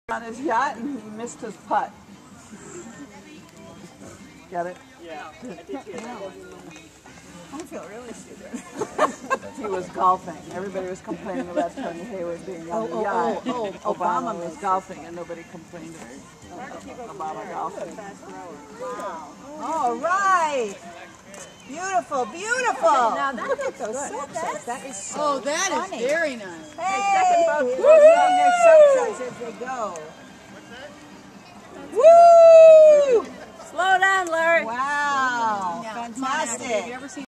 ROUNDING THE BEND.MP4
THE REGATTA ROUNDS THE BEND IN LAKE WOODROCK ON JULY 3, 2010